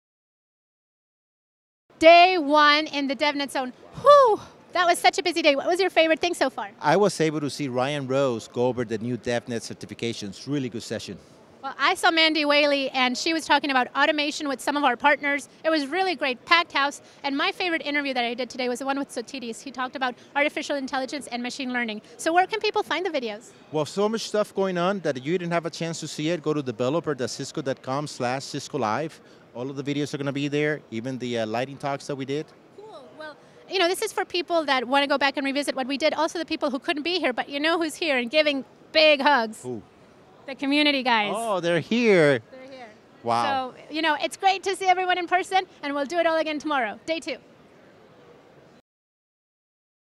Jan 27, 2020 Cisco Live Barcelona 2020 - Recap Day 1
Check out what we did day 1 (Tuesday Jan 27) at Cisco Live Barcelona.

Silvia Spiva and Julio Fernandez